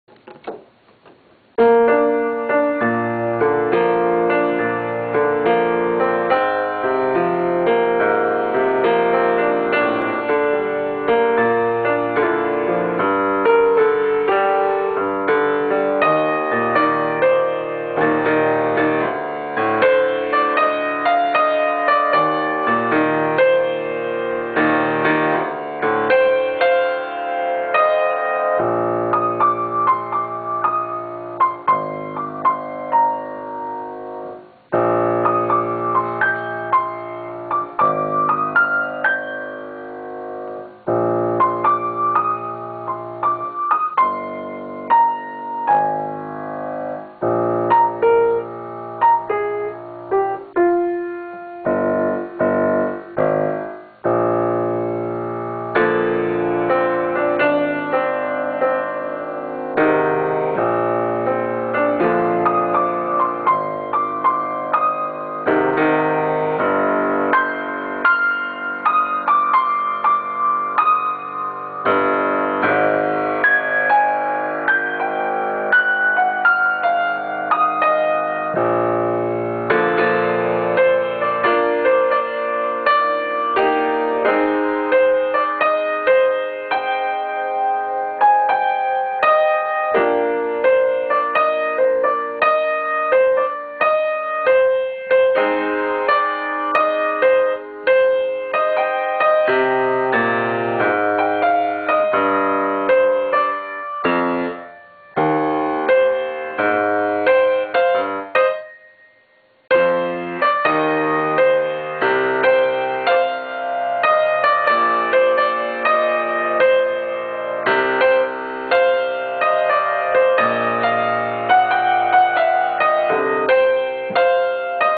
Watch me improvisation for a while
I'll probably do another one... probably 5 minutes long or so.  I actually ran out of battery power for my camera on this one.  2 minutes was all i could get.